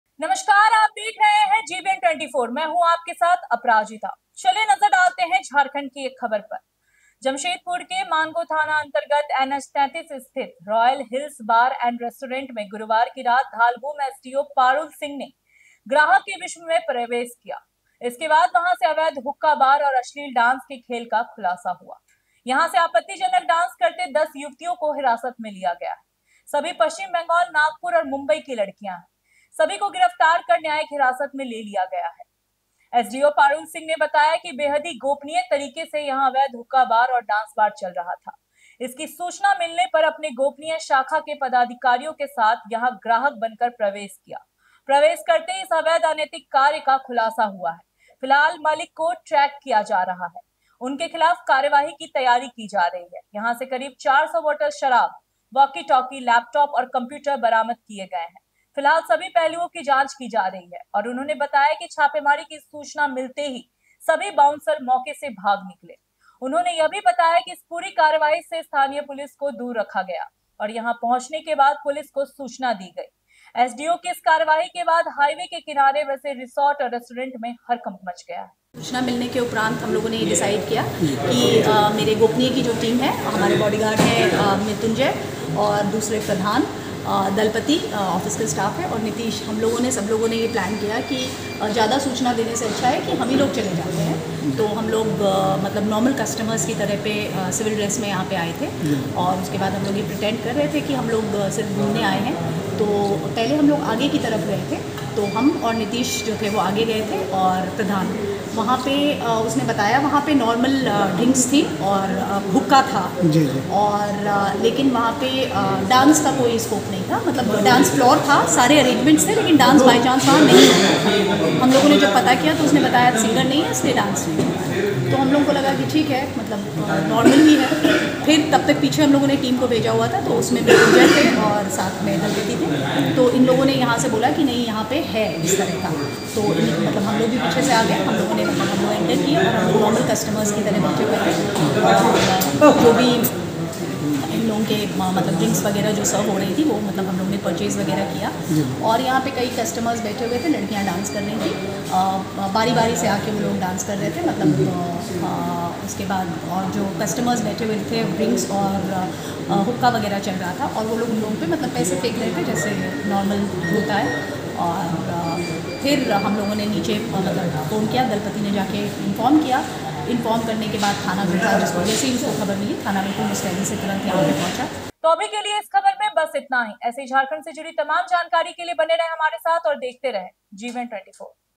Jamshedpur: गैरकानूनी हुक्का बार और अश्लील डांस का खुलासा | Gbn24
Gbn24 में अवैध हुक्का बार और अश्लील डांस के खेल का हुआ खुलासा। इस वीडियो में हम अवैध हुक्का बार के बारे में चर्चा करेंगे। यदि आप जानना चाहते हैं कि कैसे अवैध हुक्का बार और अश्लील डांस के खेल होते हैं, तो इस वीडियो में देखें! Gbn24 आपको इस खुलासे के बारे में जानकारी देगा। Gbn24 ने किया अवैध हुक्का बार और अश्लील डांस के खेल का हुआ खुलासा । यहाँ जानिए क्या है सच्चाई।

Gbn24 News | Gbn24 | Hindi News | Gbn24 News Live